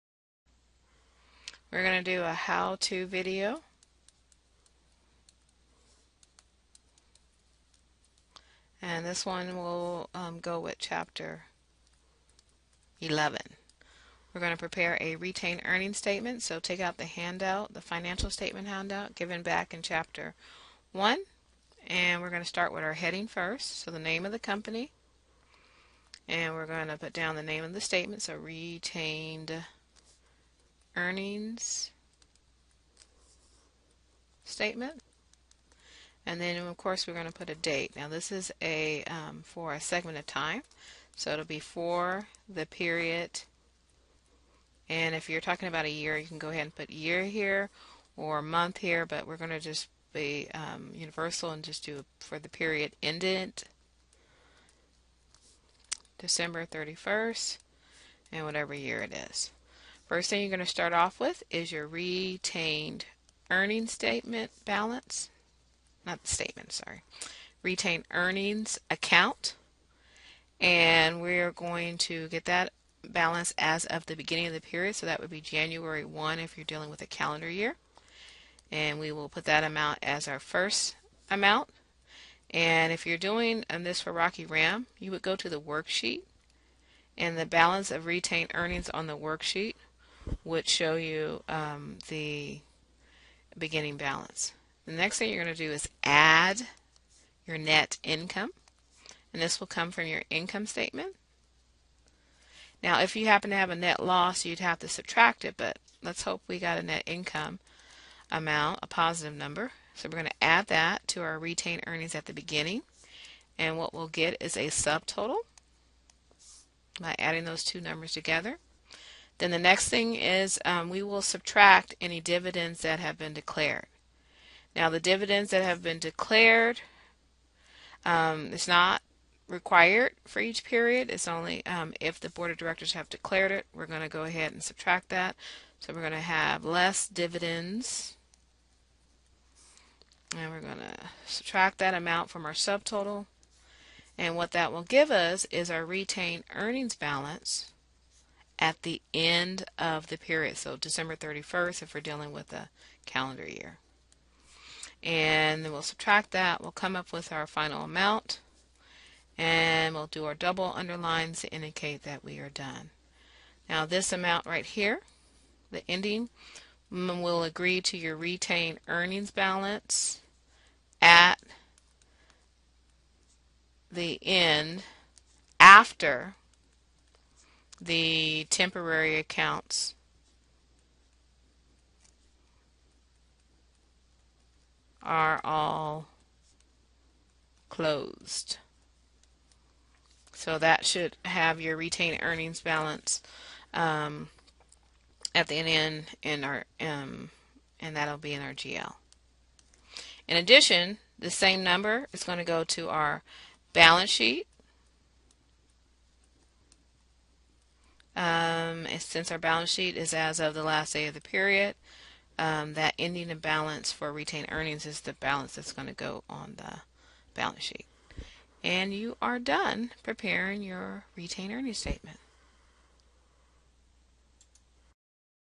How to Prepare a Retained Earnings Statement
How to prepare a basic retained earnings statement for chapter 11 and Rocky Ram practice set assigned in fall 2015.